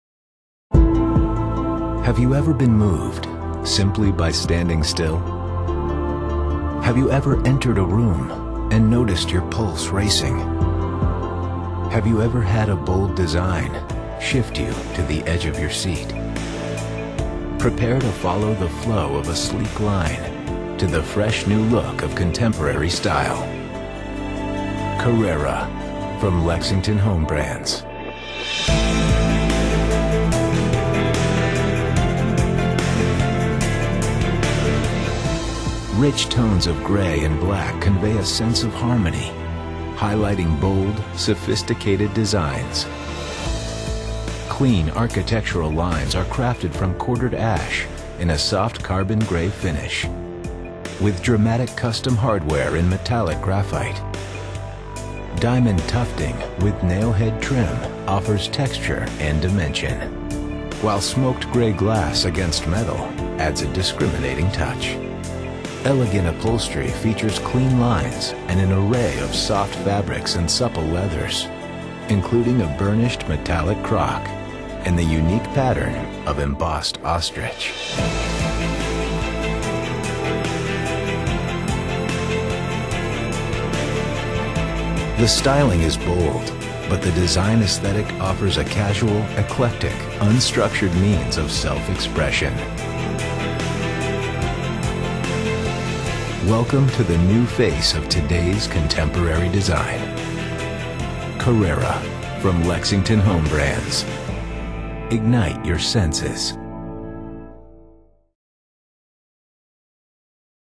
Carrera by LEXINGTON
Clean organic lines define today’s contemporary design, underscored by dark sophisticated finishes, dramatic hardware and rich textural fabrics. The look offers a structured perspective that feels modern, fresh and functional. Designs in Carrera feature styling that speaks to creating an inviting, uncomplicated, self-expressive home environment. The sophisticated carbon gray finish, sleek metallic graphite hardware and smoked gray glass fashion an unforgettable look, and a new interpretation of chic contemporary design.